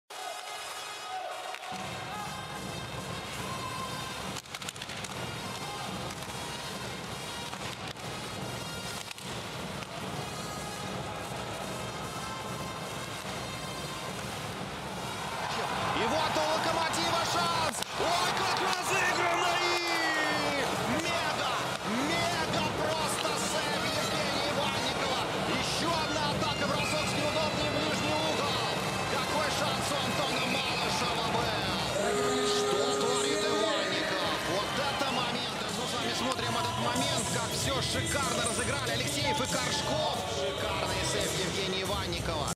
Ivannikov marvelous stretching save
Evgeny Ivannikov stretches wide to stop Loko's odd man rush chance

Евгений Иванников уже которую неделю создает контент для топ-10